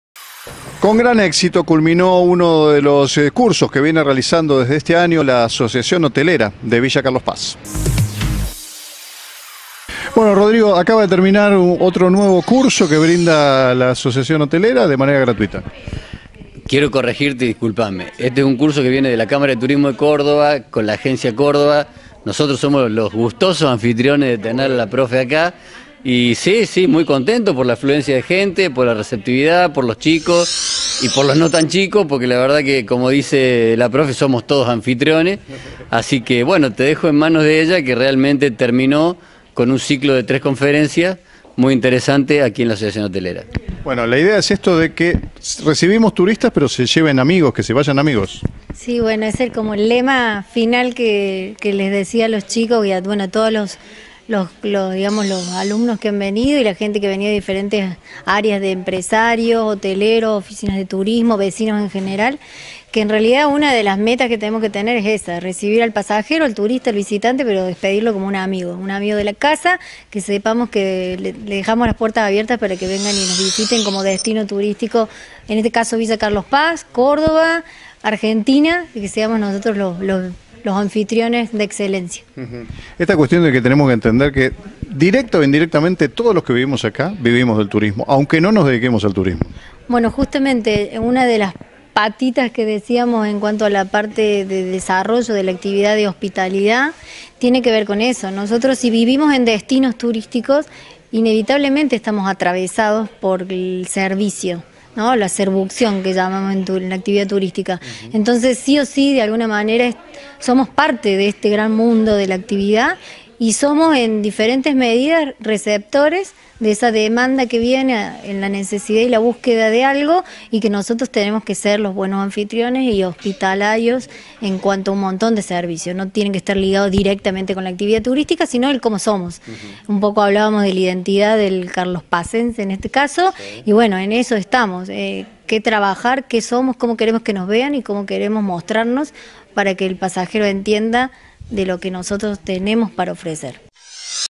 168 Horas TV - Jornada de Hospitalidad en Ashoga
En la tarde del miércoles 23 de mayo se desarrolló la “Jornada de Formación de Hospitalidad con el Visitante 2018”, organizada por la Agencia Córdoba Turismo S.E.M. y la Cámara de Turismo de Córdoba que integra la Asociación hotelera de Carlos Paz.

En ese marco, se brindaron herramientas conducentes a afianzar los conceptos básicos de hospitalidad, promoviendo la cordialidad y destacando la importancia de los anfitriones o primeros informantes del turista.

“Una de las metas que tenemos es recibir al pasajero, turista o visitante y despedirlo como un amigo de la casa; que sepamos que le dejamos la puerta abierta para que nos visite como destino turístico, en este caso Villa Carlos Paz, y que seamos nosotros los anfitriones de excelencia”, expresó la capacitadora, Romina Leguizamón.

De acuerdo a este enfoque, “tenemos que ser hospitalarios en cuanto a un montón de servicios, no solo relacionados con la actividad turística sino también ligados a nuestra identidad, cómo queremos que nos vean y cómo mostrarnos para que el pasajero vea lo que tenemos para ofrecer”.

A su vez, el presidente de ASHOGA, Lic. Rodrigo Serna, manifestó su conformidad ante la numerosa cantidad de asistentes, haciendo hincapié en el próximo evento de importancia para la provincia, el Congreso Internacional de la Lengua Española. “Es fundamental que nosotros como anfitriones seamos no solamente correctos con la lengua sino que seamos los mejores posibles porque estamos vendiendo un producto turístico que nos da esta oportunidad. En este sentido, todos somos anfitriones”, puntualizó.